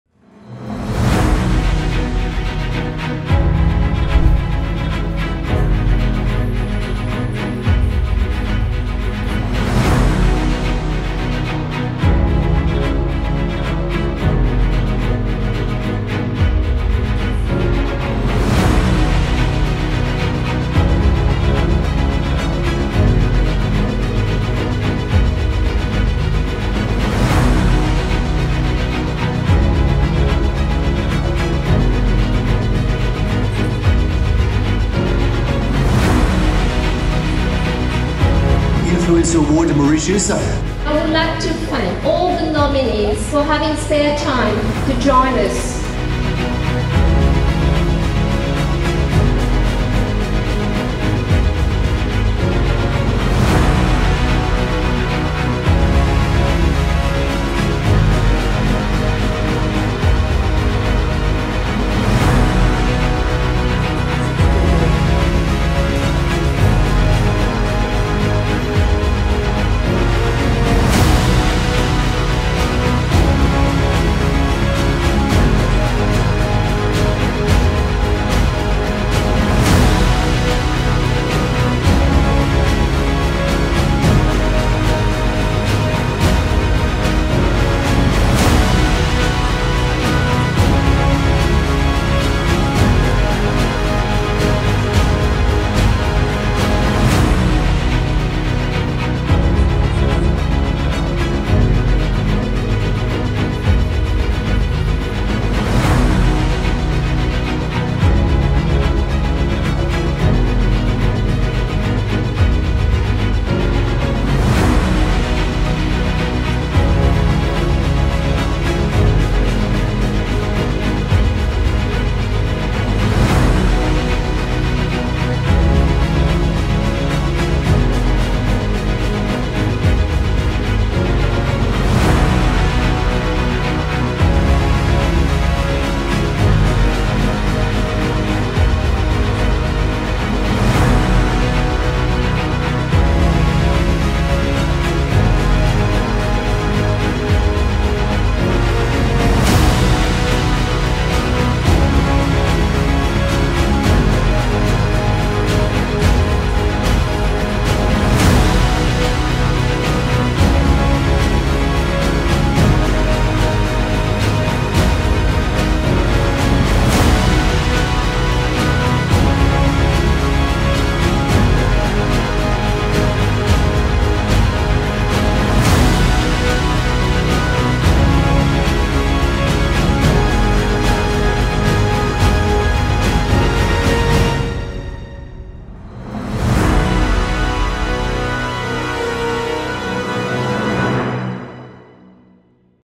Influencers Awards Mauritius Ceremony 2023
Until the next, here's a glimpse of the 2023 Influencers Awards Mauritius Ceremony.

Deepest thanks to our nominees, partners, juries and team.  We wouldn't have made it without your support.